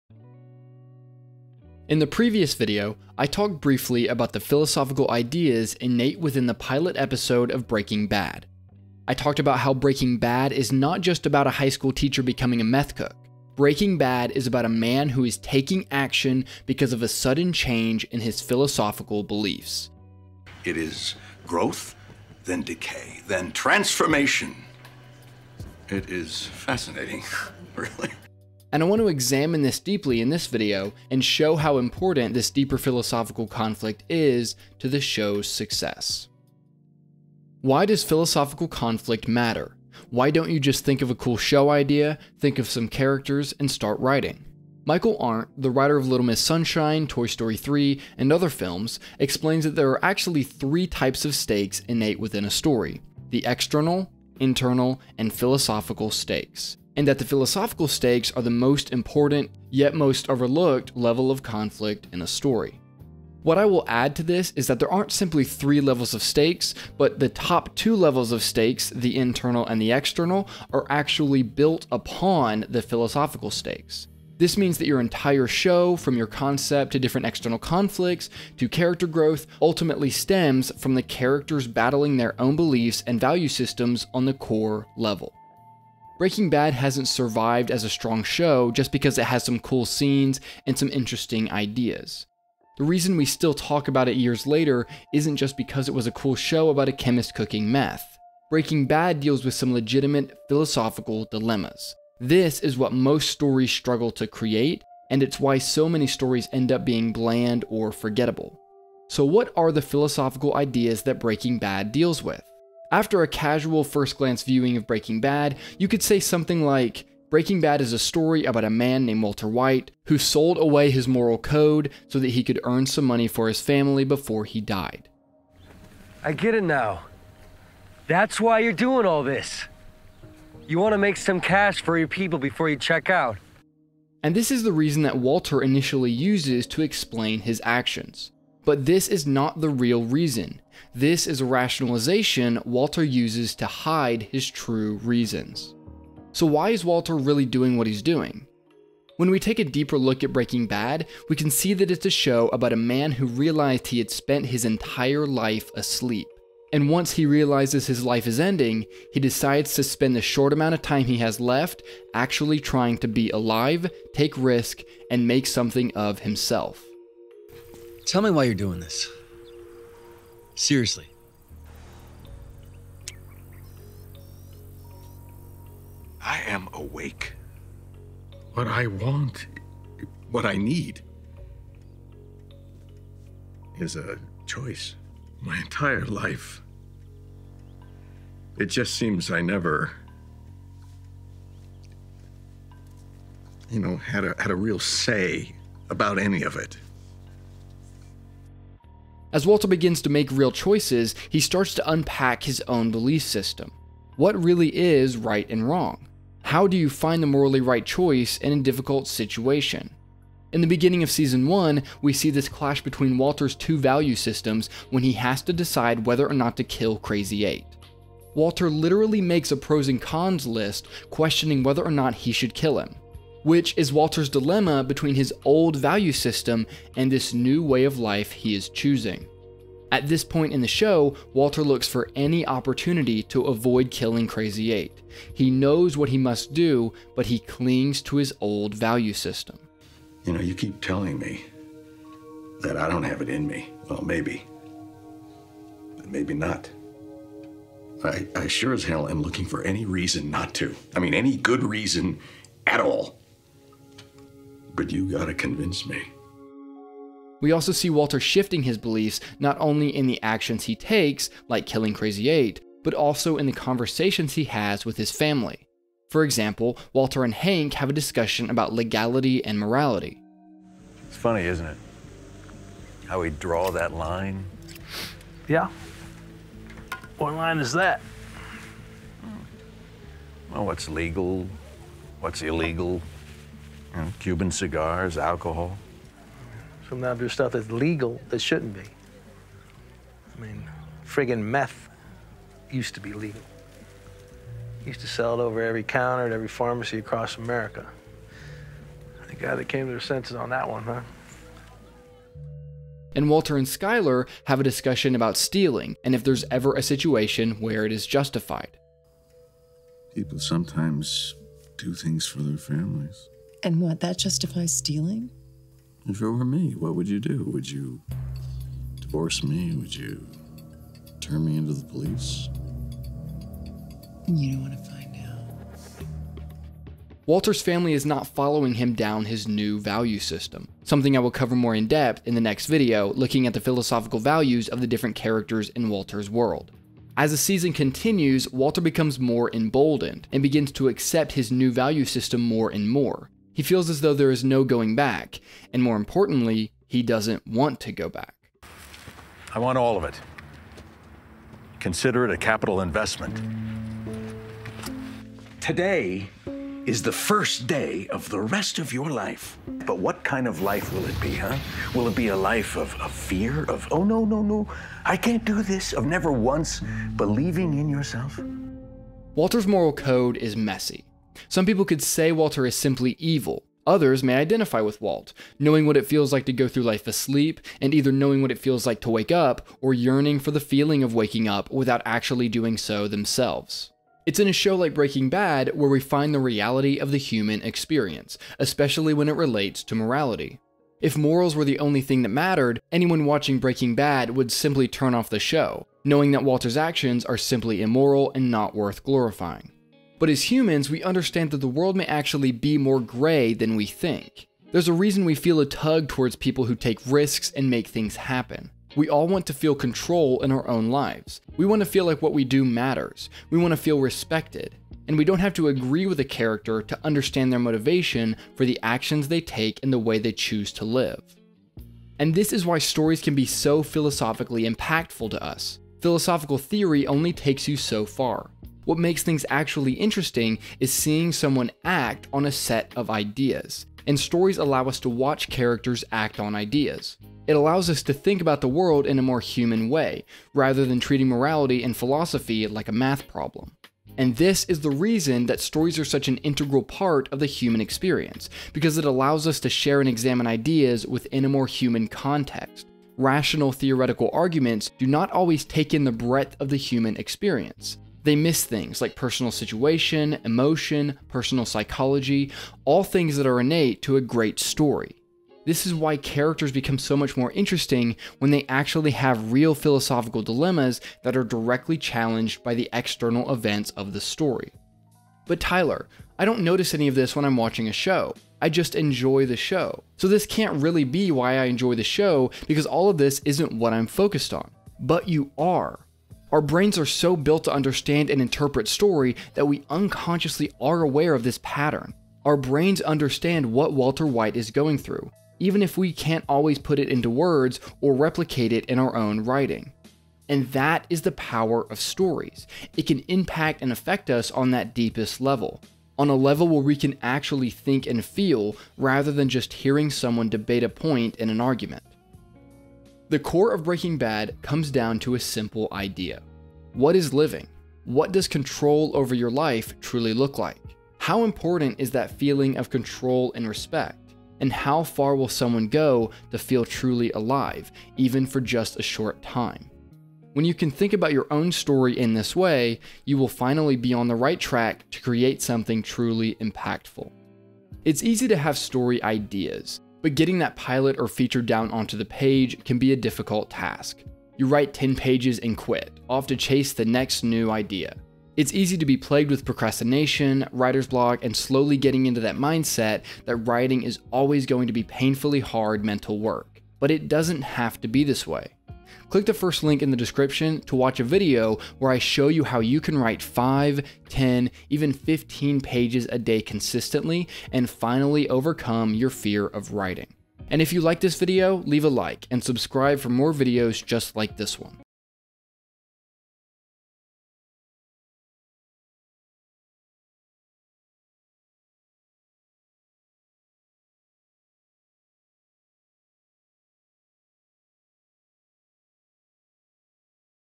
Building a TV Series Episode 2: Crafting Philosophical Conflict
In this episode, I take a look at philosophical conflict and how to apply it to a long form series. Breaking Bad is all about character change over time, and the bedrock of character change is philosophical conflict.

Shoot me a DM!